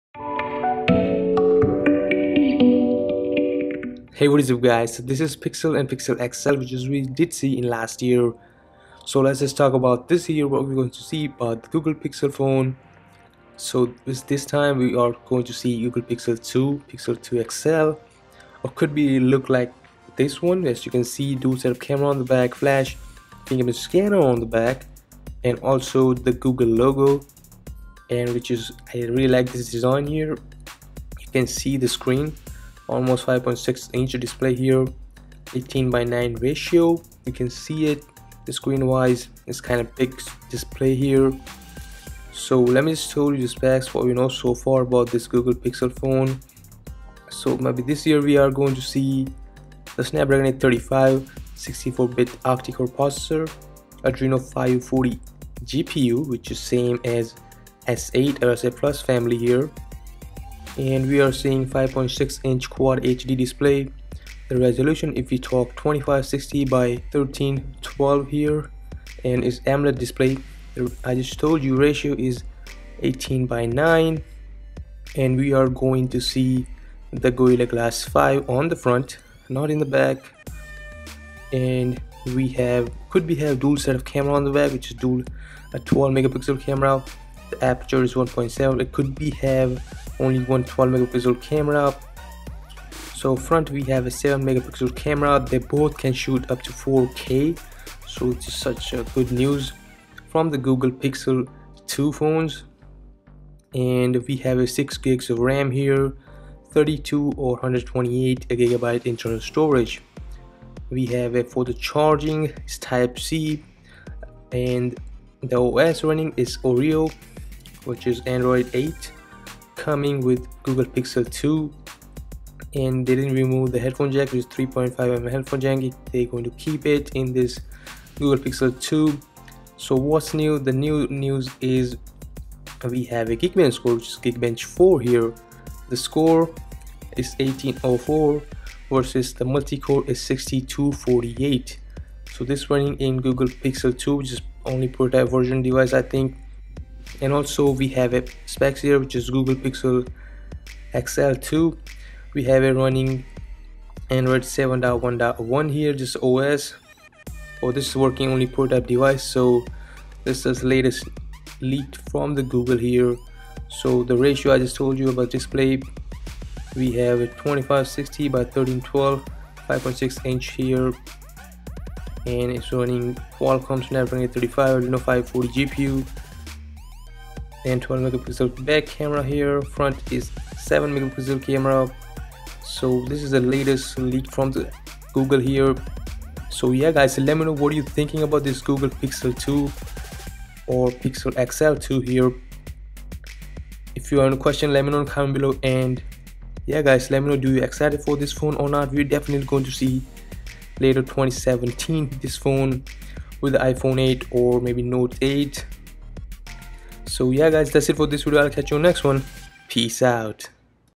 GOOGLE PIXEL 2 THE BEST OF ANDROID !!!
Google Pixel 2 Leaks. New Amazing display and Pure stock Android. Best of both worlds?

This concept smartphone is fueled by the Snapdragon 835 64-bit octa-core processor, along with the Adreno 540 GPU for graphics processing. The real Google Pixel 2 handset will probably be fueled by this very same SoC, in case you were wondering. This concept phone features a 5.6-inch QHD (2560 x 1440) AMOLED display (18:9 aspect ratio) with Corning’s Gorilla Glass 5 protection on top of it. Two 12-megapixel shooters (f/1.7 aperture, 1.65um pixel size) are included on the back, while the phone packs in 6GB of RAM and 32GB / 128GB of expandable internal storage. Fast charging is also listed by the source, so is wireless charging. A Type-C USB port is also included in this package, of course, and so is Android 8. Interestingly enough, a 3.5mm headphone jack has also found its place here, and many people are hoping that the real Google Pixel 2 will also sport that audio jack. The device tested by Geekbench also scored 1,804 in single-core testing and 6,248 in multi-core.

▶︎(Give your friends $5 to spend at Amazon.)(▶︎▶︎SUMIT3611N◀︎◀︎)